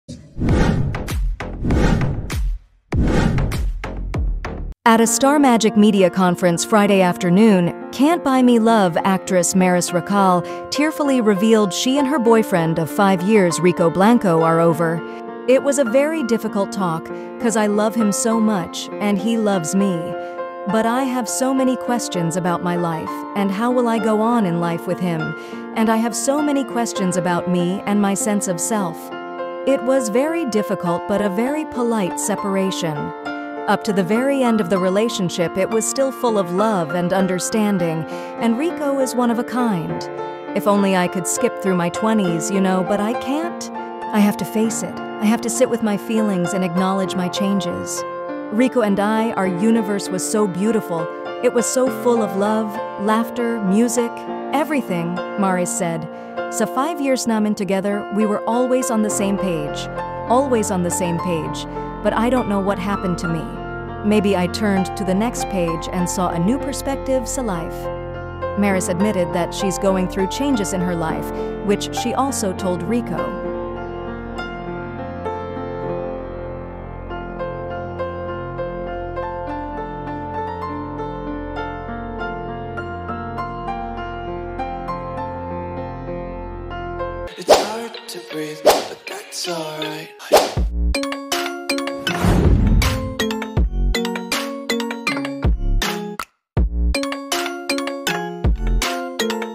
MARIS RACAL HUMAGOLGOL NANG IYAK CONFIRMM BREAK UP WITH RICO BLANCO
Hiwalay na nga ang mag kasintahang Maris Racal ang Rico Blanco.Kapamilya actress Maris Racal confirmed on Friday that she and her boyfriend, singer-composer Rico Blanco, have broken up.

"Rico and I are over,"she said after she was asked in the Star Magic Spotlight event how she weighs her personal life and fame with on-screen partner Anthony Jennings after their stint in "Can't Buy Me Love."

"I'm so scared, I'm really scared," Racal emotionally told the press. "Ang dami kasing tanong about him and I am tired of smiling and nodding."

"I'm so scared because If I announce it, it's real," she added.